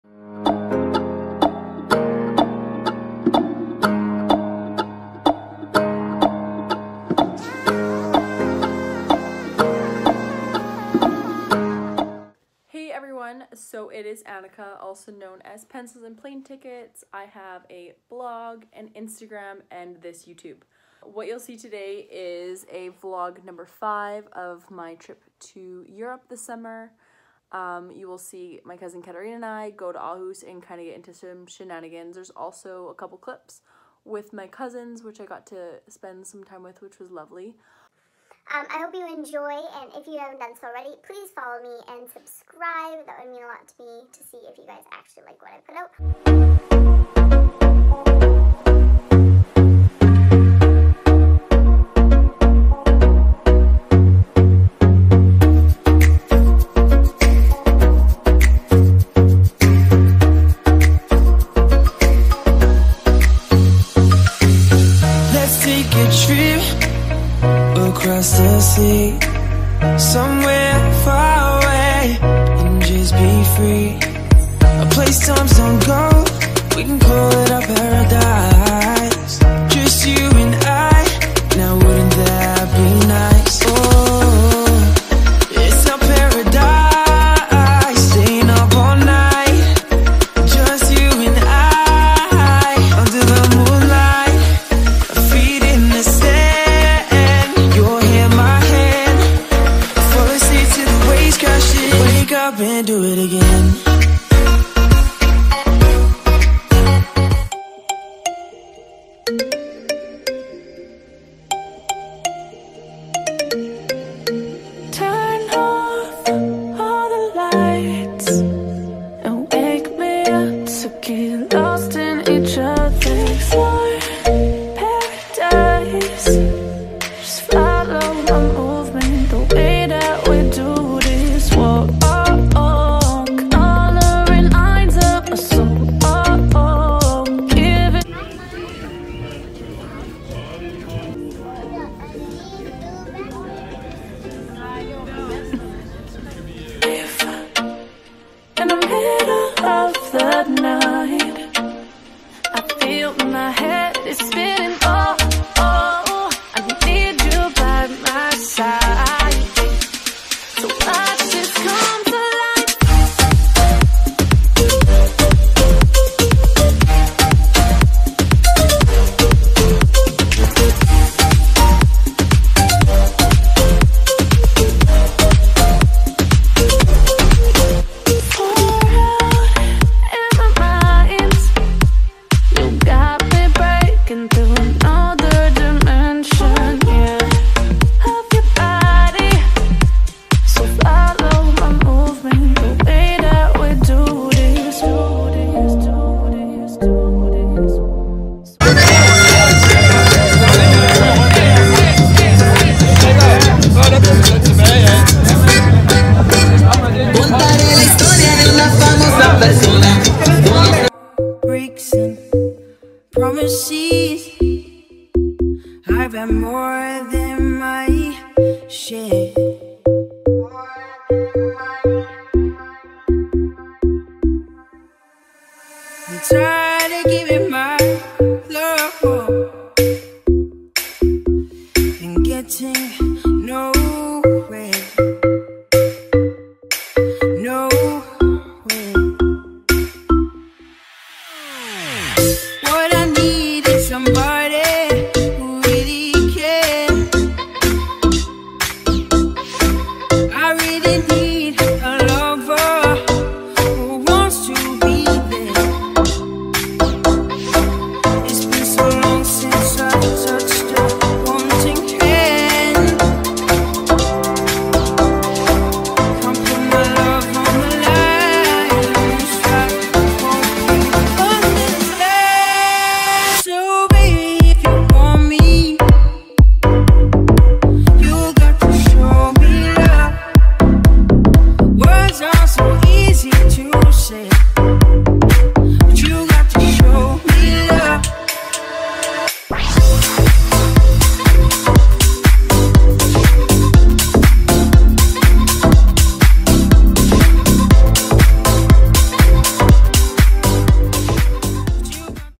cope vlog 5: aarhus shenanigans
Come along to Aarhus, Denmark with me and Kat, we took a bus and ship combo on Kombardo. We saw some sights, had some laughs and even found ourselves at a tall ship race ;)

all footage shot on an iPhone 8 plus!

xoxo